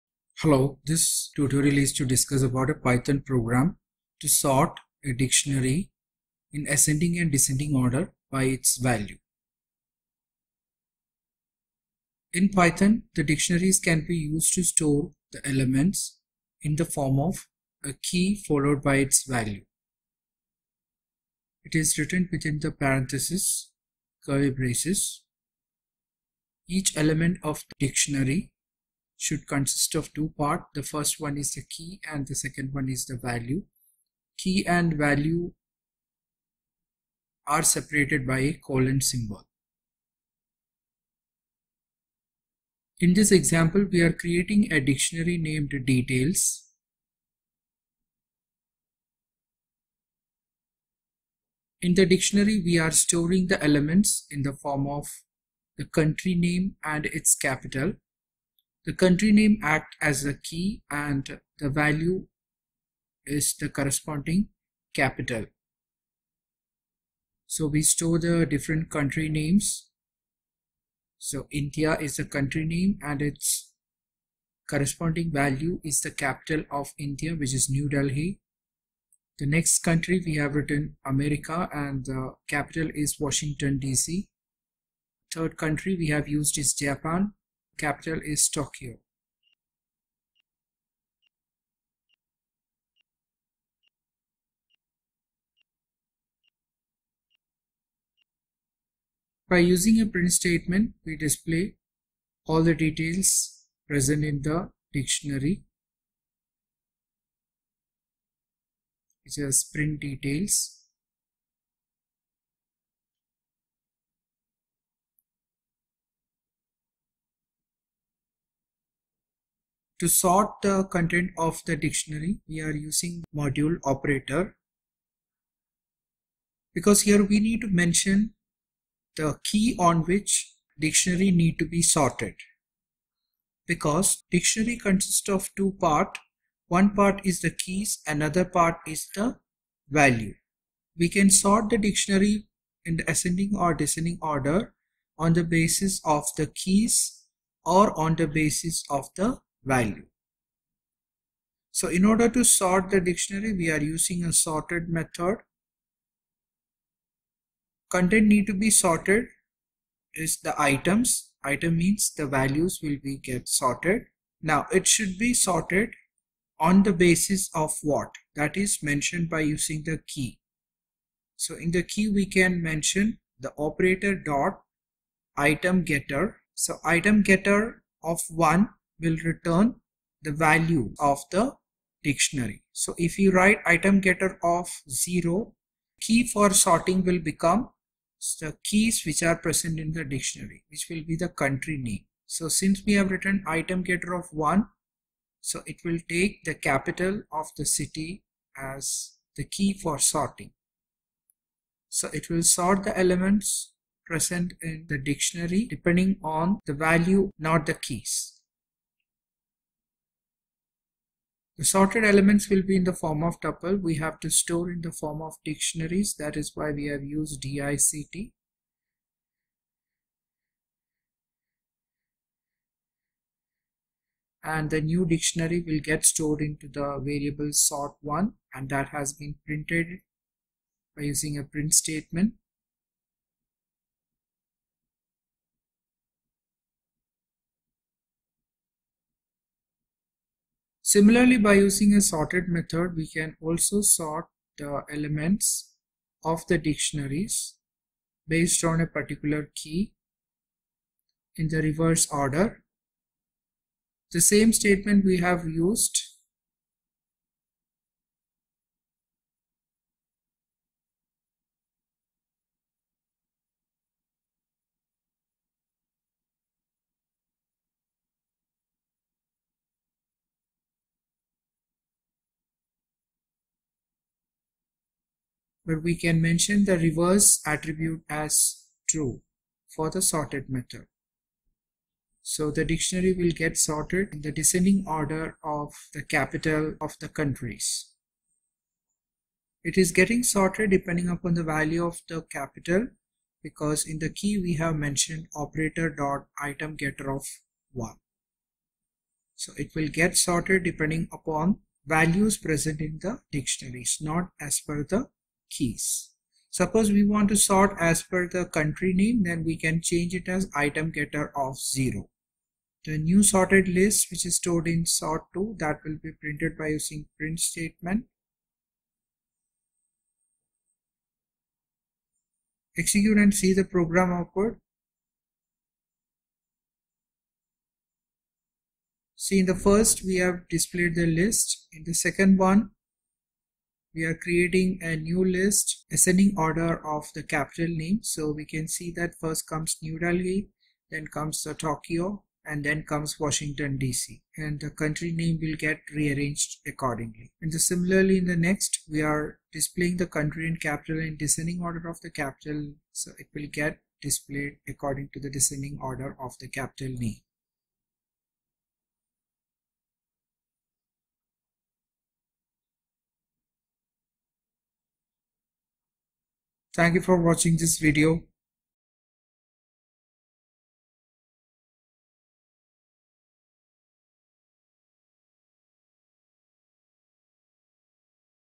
Python Program - sort dictionary
Write a Python script to sort (ascending and descending) a dictionary by value.